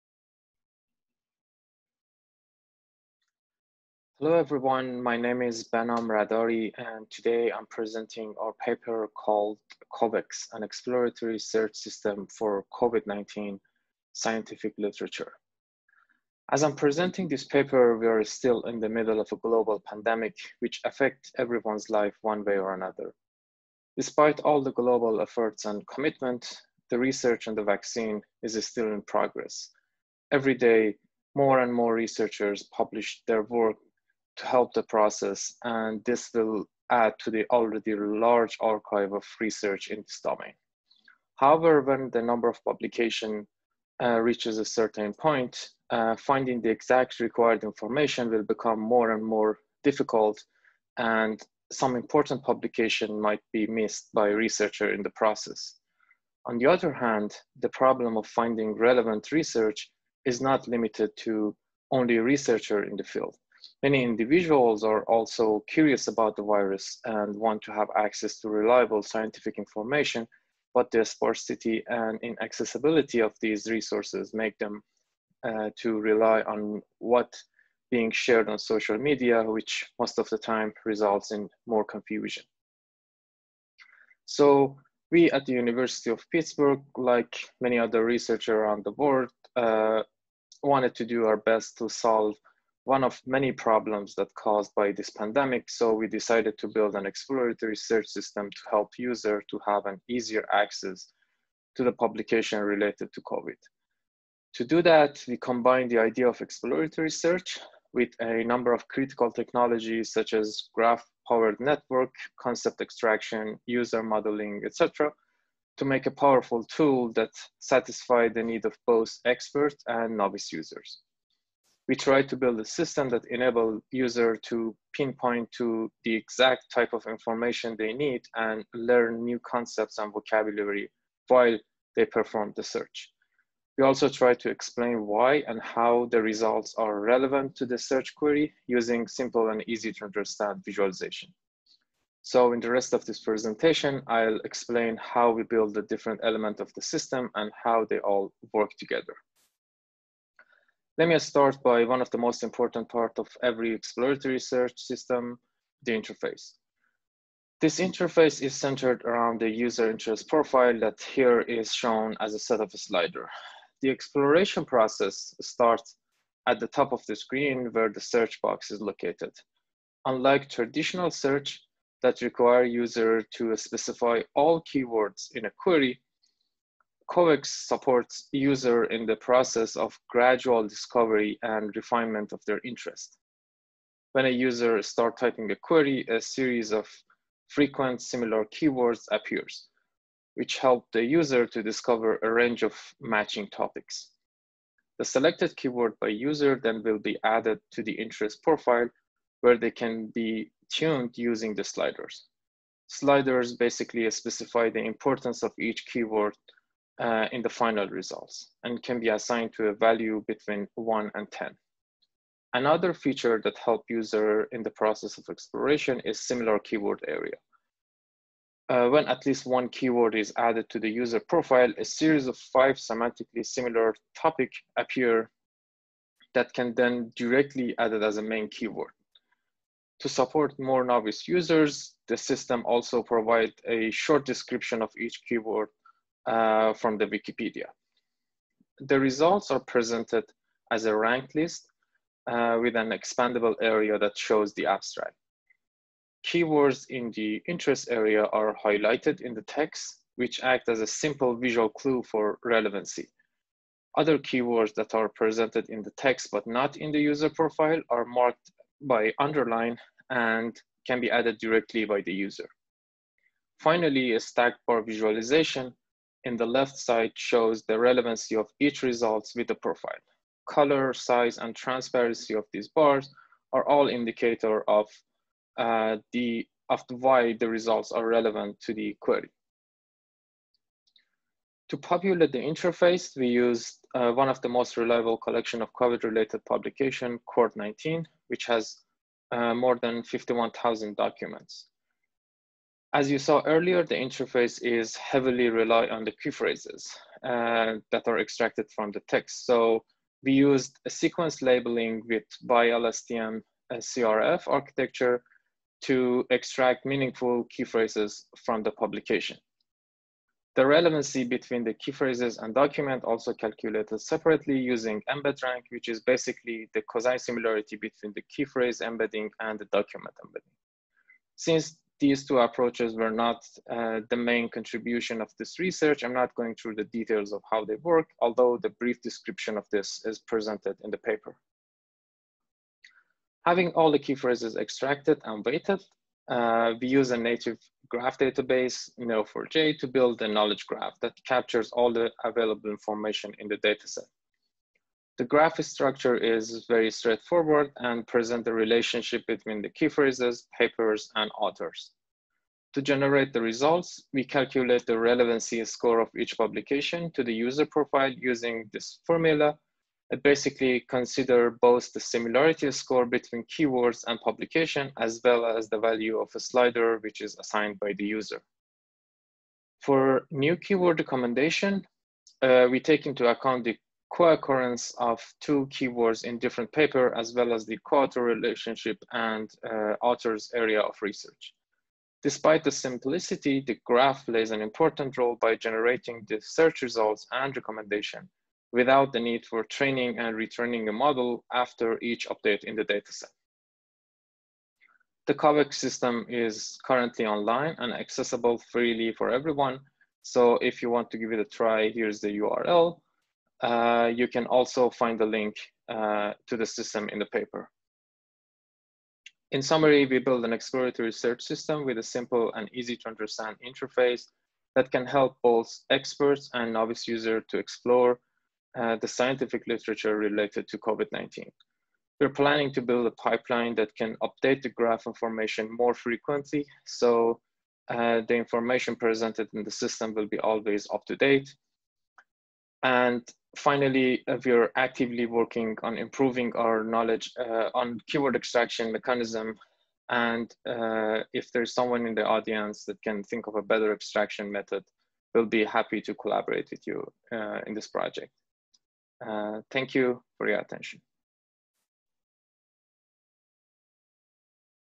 CovEx: An exploratory search system for COVID-19 scientific literature
Behnam Rahdari, Peter Brusilovsky, Khushboo Thaker and Hung K. Chau. CovEx: An exploratory search system for COVID-19 scientific literature. ACM SIGKDD 2020 epiDAMIK workshop. Poster paper.